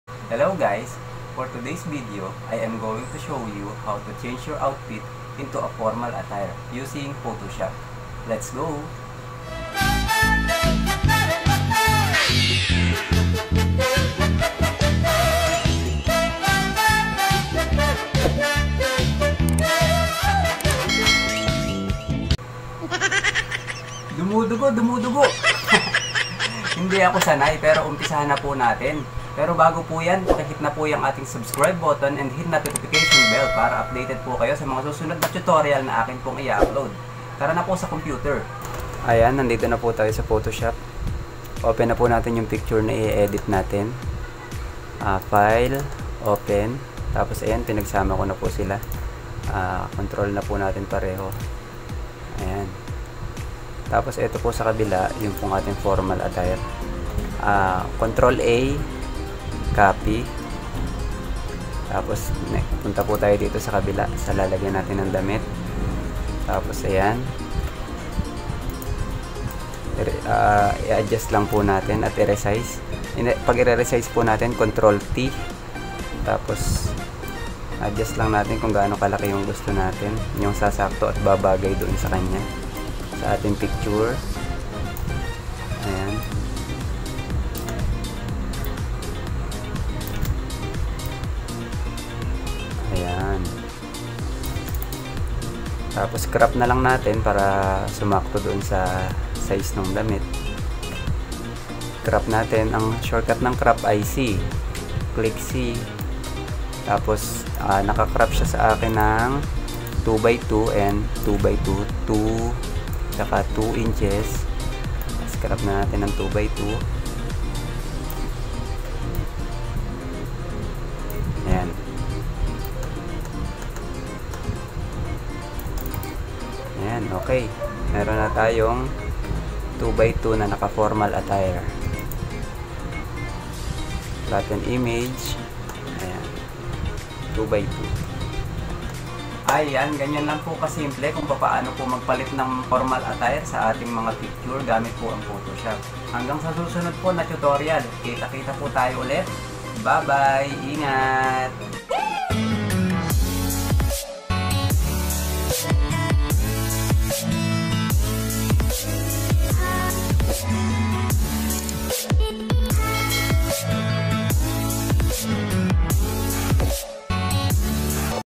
Photoshop Tutorial | How to Change suit or coat in Photoshop | E&M Hermoso
Photoshop Tutorial |  How to Change suit or coat in Photoshop

This video will provide you a tutorial on how to change suit or coat in Photoshop